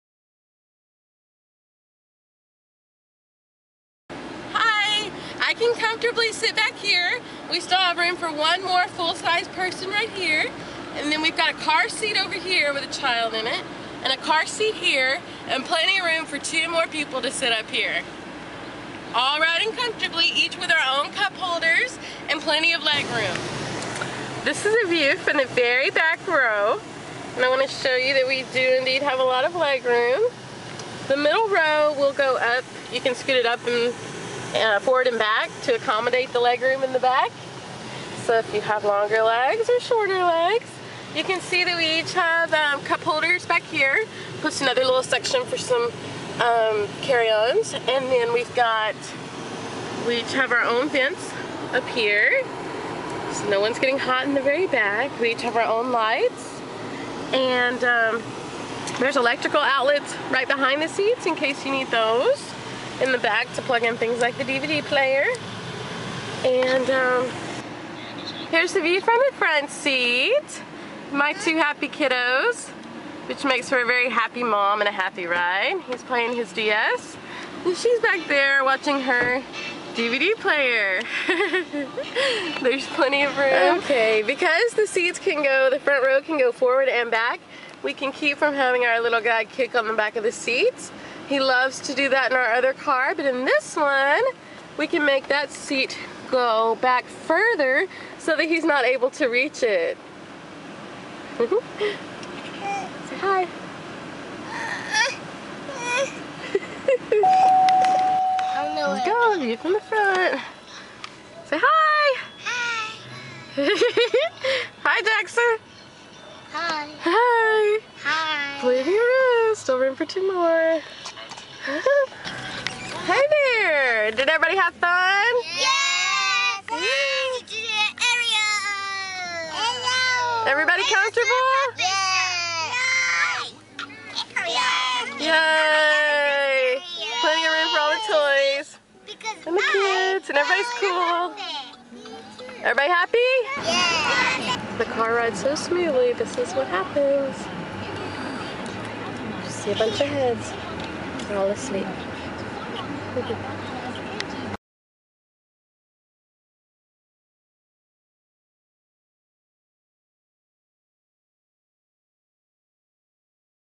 chevy traverse on a road trip with kids
Can the traverse really hold that many?